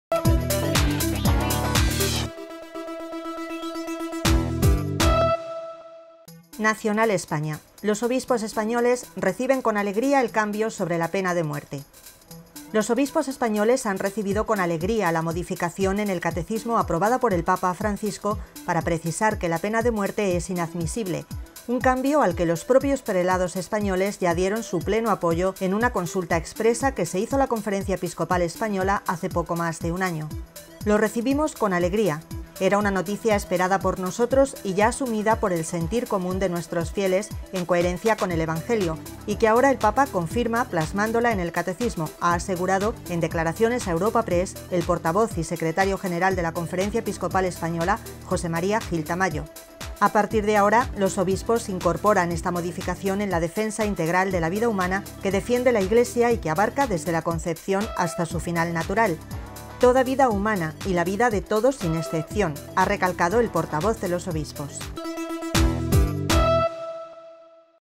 Vídeo Noticia: Los obispos españoles reciben «con alegría» el cambio sobre la pena de muerte
Los obispos españoles han recibido con «alegría» la modificación en el Catecismo aprobada por el Papa Francisco para precisar que la pena de muerte es «inadmisible», un cambio al que los propios prelados españoles ya dieron su «pleno apoyo» en una «consulta expresa» que se hizo a la Conferencia Episcopal Española (CEE) hace poco más de un año.